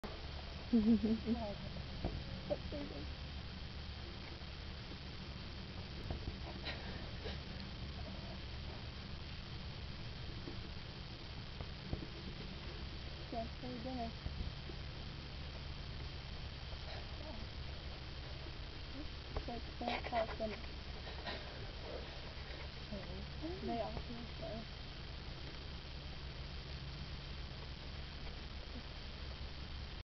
Black-footed Ferret Dance
I used to work on the ferret project in Aubrey Valley, Arizona. (yes these are the native and endangered ones not the ones in the pet store) This is in one of the preconditioning pens. Many ferrets will do this strange slinky-like dance.